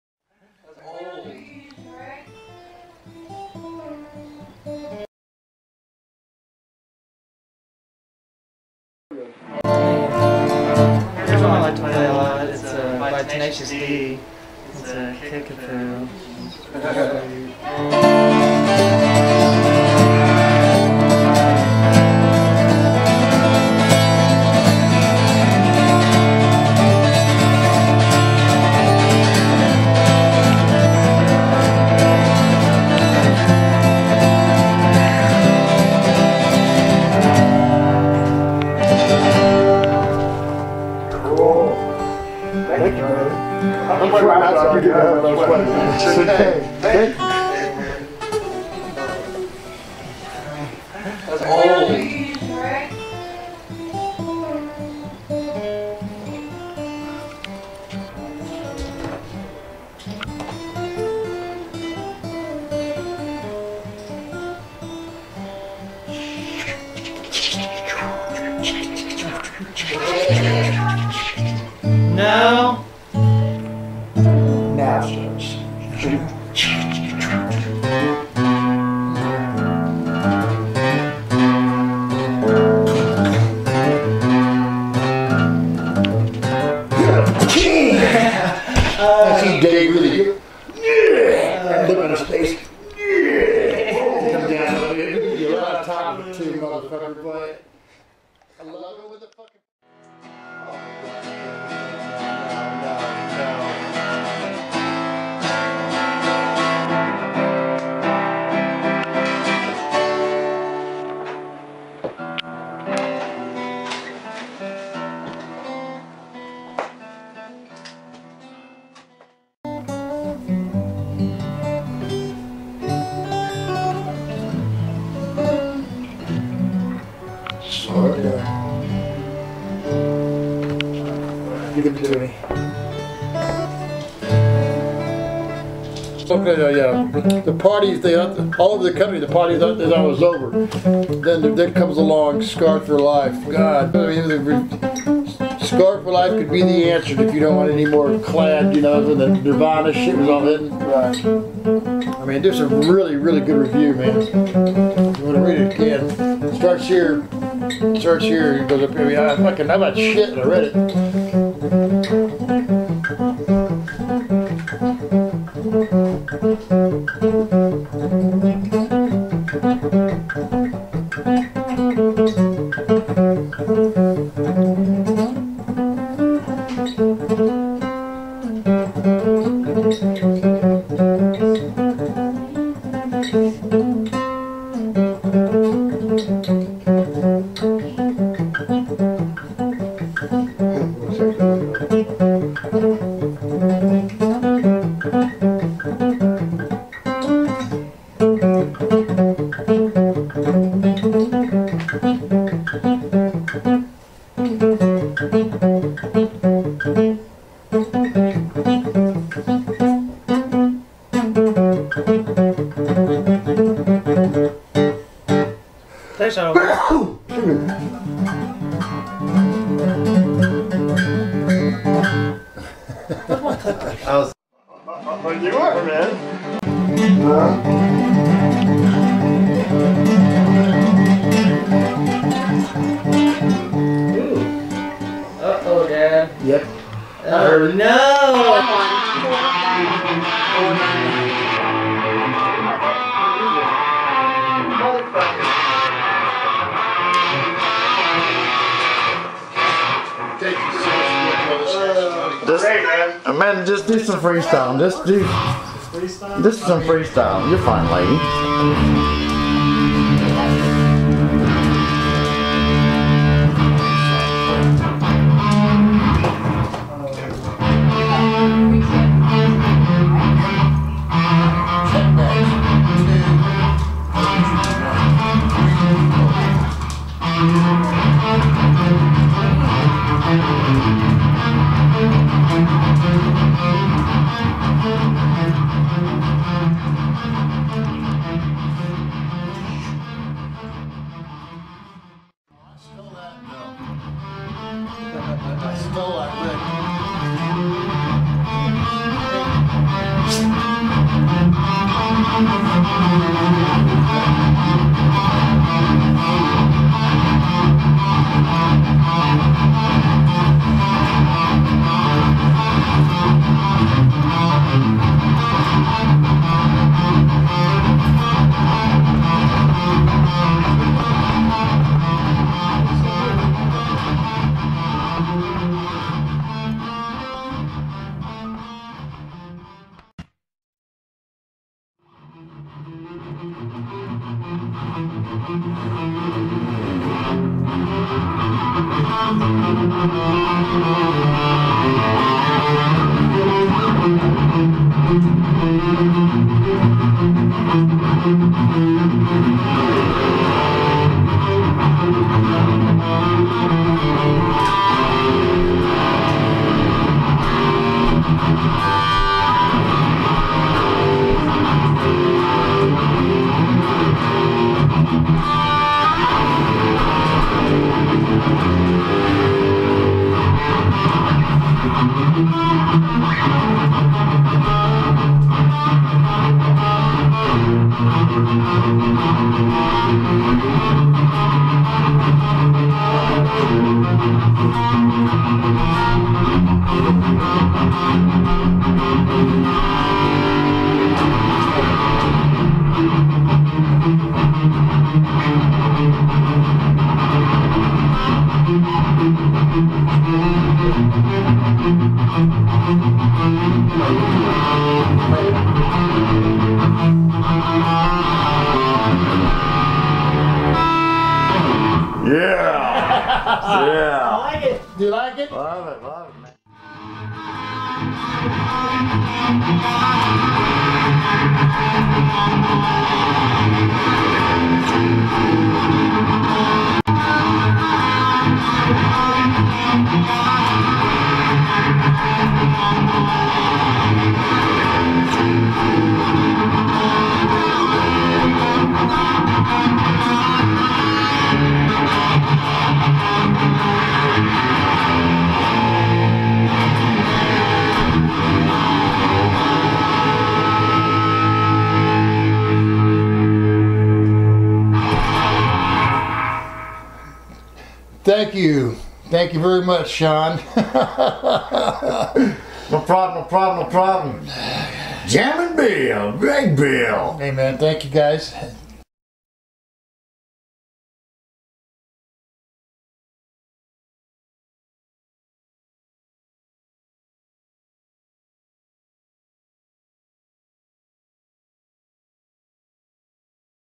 Back in the Day Father and son jamming away
Back in the day father Gregory Jacob Bill and son was Jamming away One less then there was in that room R.I.P.